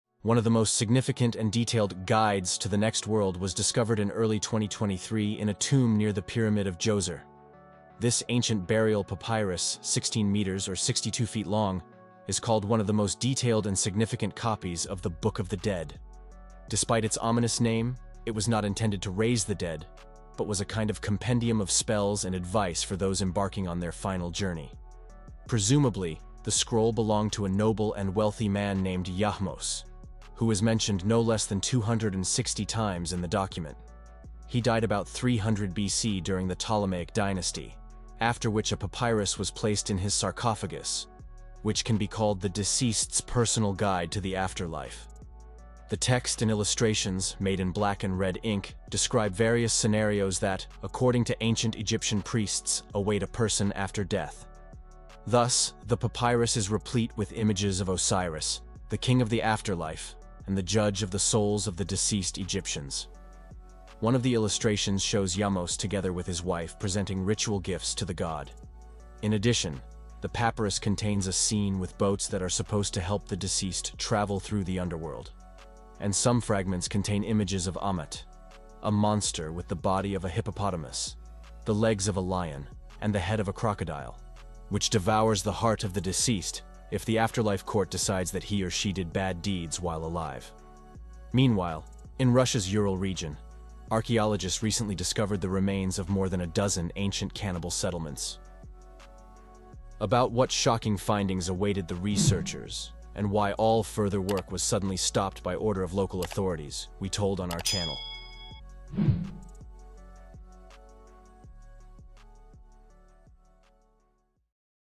The "Book of the Dead" Revealed Ancient Mysteries
One of the most significant and detailed "guides" to the next world was discovered in early 2023 in a tomb near the pyramid of Djoser.
This ancient burial papyrus 16 meters or 62 feet long is called one of the most detailed and significant copies of the "Book of the Dead". Despite its ominous name, it was not intended to raise the dead, but was a kind of compendium of spells and advice for those embarking on their final journey.